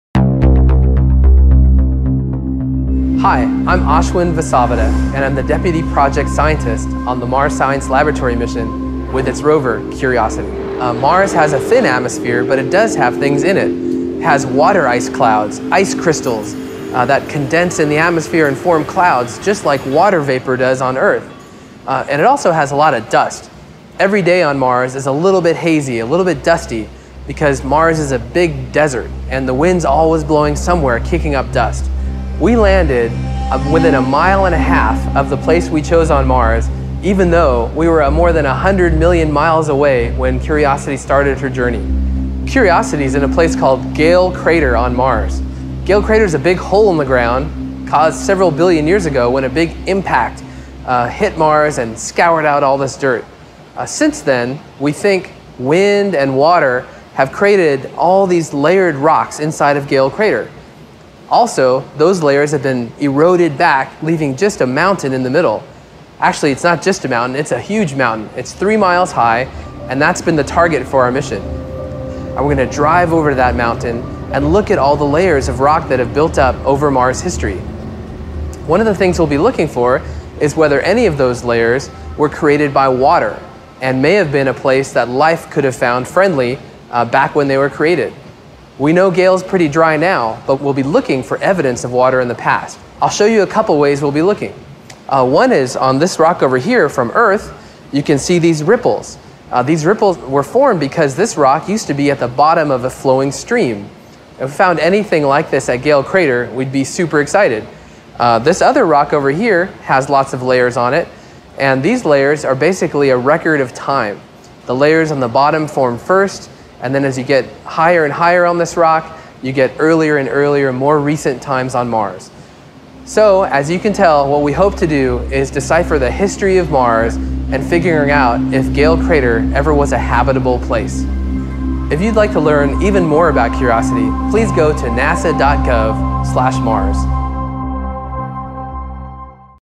The Surface of Mars - presented by Angry Birds Space
Anyone who has played Angry Birds Space knows that the game contains a satellite that links to a NASA mobile web page that educates kids on the effects of microgravity and that Angry Birds celebrated the landing of NASA's Curiosity Mars rover with Mars-themed levels in Angry Birds space. What may not be known is that NASA and Rovio, the developers of Angry Birds, have teamed up to provide educational programming about space on the web. Just this month, Rovio also announced that it has teamed up with CERN to teach Finnish children about physics through Angry Birds Playground.